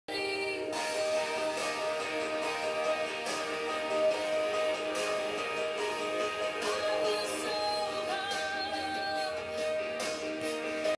Turtle's Gone Wild
You don't see this little guy for months .. and then all of the sudden, "HE's OFF!"